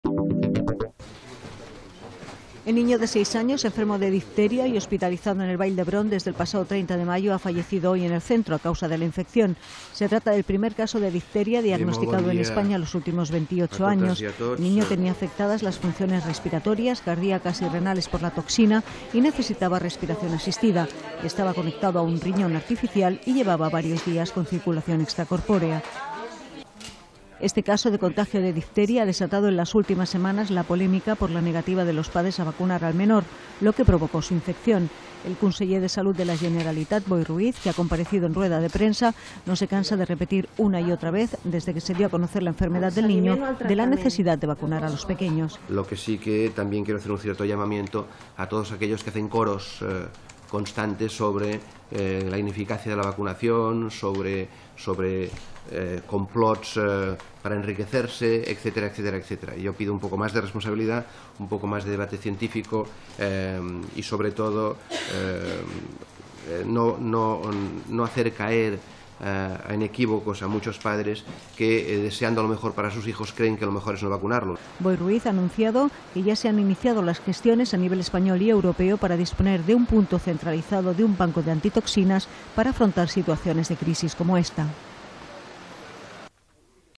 Fallece el niño de seis años afectado de difteria
Barcelona, 27 jun (EFE).- El niño de 6 años enfermo de difteria y hospitalizado en Vall d'Hebrón desde el pasado 30 de mayo, ha fallecido hoy en el centro a causa de la infección.
Se trata del primer caso de difteria diagnosticado en España en los últimos 28 años.
El niño tenía afectadas las funciones respiratorias, cardíacas y renales por la toxina de la difteria, y necesitaba respiración asistida, estaba conectado a un riñón artificial y llevaba varios días con circulación extracorpórea.
Este caso de contagio de difteria ha desatado en las últimas semanas la polémica por la negativa de los padres a vacunar al menor, lo que provocó su infección.
El conseller de Salud de la Generalitat, Boi Ruiz, que ha comparecido en rueda de prensa no se cansa de repetir una y otra vez desde que se dio a conocer la enfermedad del niño, de la necesidad de vacunar a los pequeños.
Boi Ruiz ha anunciado que ya se han iniciado las gestiones a nivel español y europeo para disponer en un punto centralizado de un banco con antitoxinas para afrontar situaciones de crisis como ésta. Palabras clave: efe,salud,difteria, niño,olot